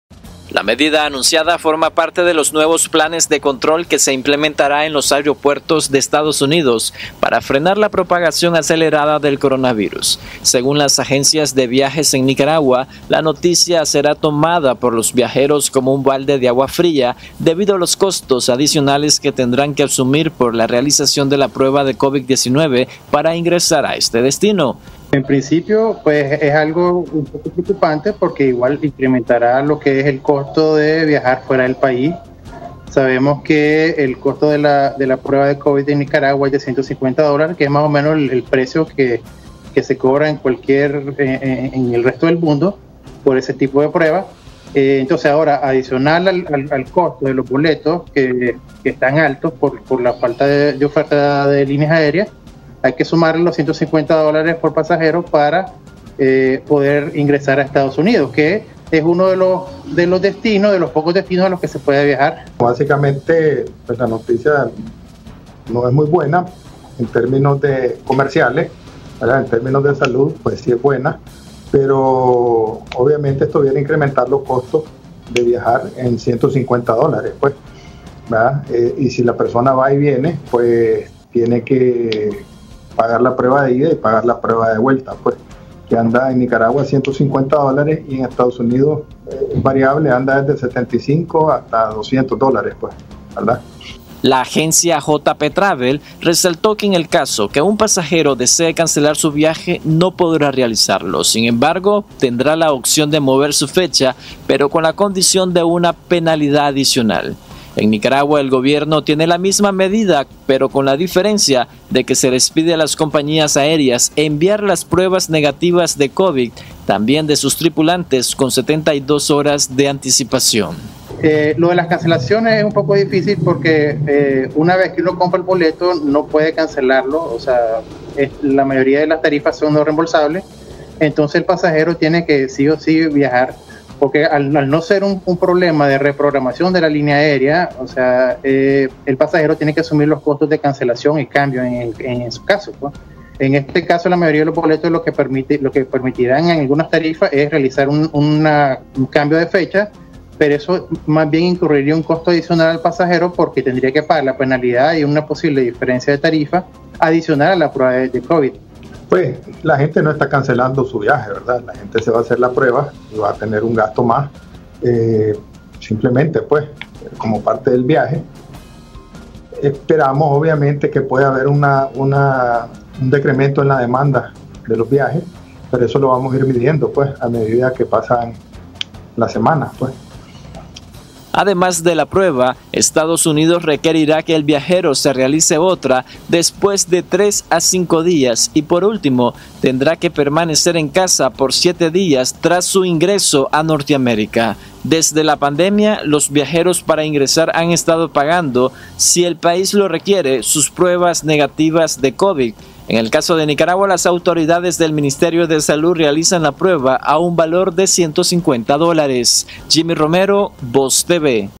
Estados Unidos comenzará a pedir prueba de Covid-19
La orden fue emitida este martes por el director de los Centros de Control y Prevención de Enfermedades (CDC), Robert Redfield, en coordinación con la Casa Blanca.